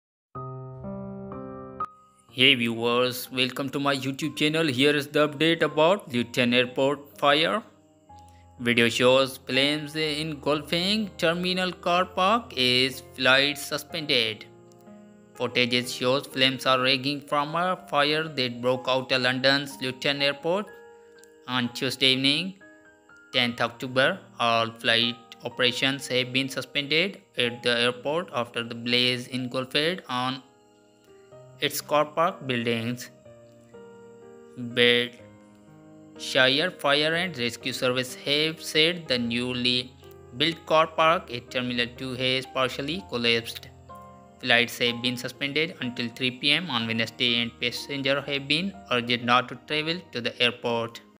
Luton Airport car park fire video - Huge fire rages at Luton Airport, all flights suspended
All flight operations have been suspended at the airport after the blaze engulfed one of its car park buildings.

Footage shows flames raging from a fire that broke out at London’s Luton Airport on Tuesday evening (10 October).

Bedfordshire Fire and Rescue Service have said the newly-built car park, at Terminal 2, has partially collapsed.

Flights have been suspended until 3pm on Wednesday and passengers have been urged not to travel to the airport as “access remains severely restricted”.

Up to 1,200 vehicles may have been in the car park at the time of the fire and subsequently damaged, according to the fire services.